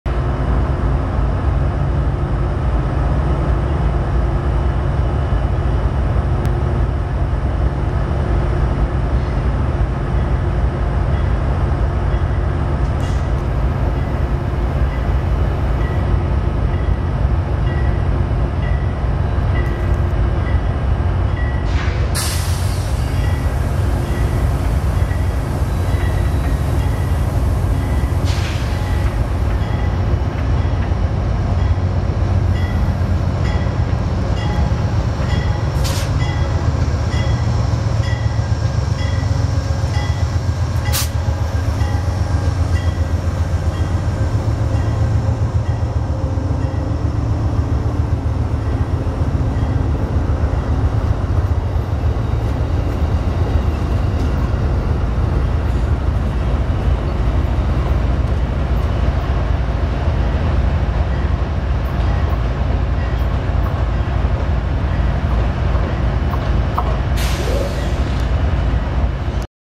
1/15/18 Preview clip: CDTX 2051 leads a block set into LAUPT
1/15/18 Preview clip: CDTX 2051 leads a block set into LAUPT

Here's CDTX 2051 leading a trio of leased Amtrak California cars  including CDTX Cabcar 8309 being brought into LAUPT for Amtrak train 769 this early afternoon.

JonRailVideos2018